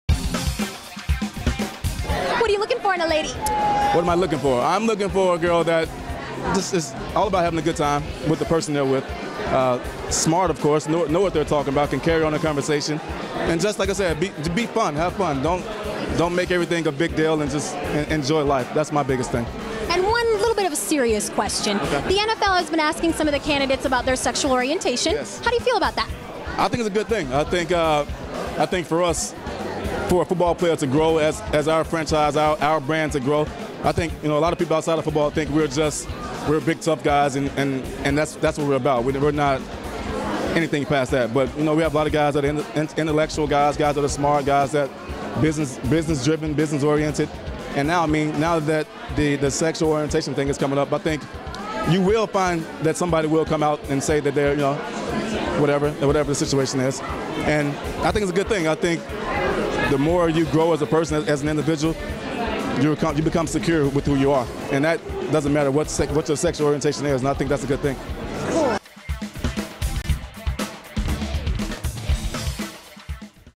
KERRY RHODES TALKS GAYS IN FOOTBALL & WOMEN | CHRISTYreports.com
NFL superstar Kerry Rhodes answers questions about football & homosexuality & what he's looking for in a woman, contradicting reports that he is gay...